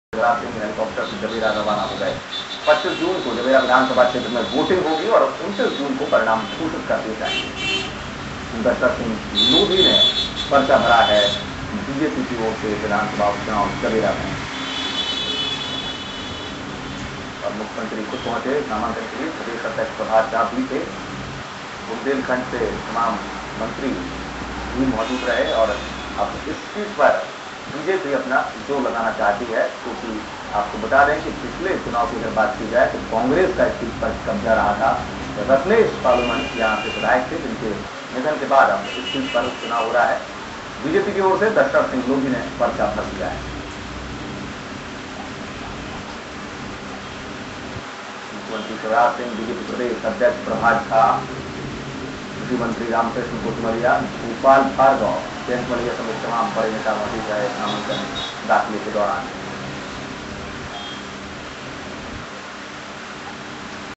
ATAL RAJENDRA JAIN SADHNA NEWS DAMOH 08-06-11 DASHRATH SINGH NE NEMANCAN BHARA.wmv
DAMOH-JABERA UPCHUNAV ME BJP KE DASHRATH SINGH NE APNA FORM BHARA, IS MOUKE PER CM SHIVARAJ SINGH SAHIT 5 MINISTER ME MOUJOOD RAHE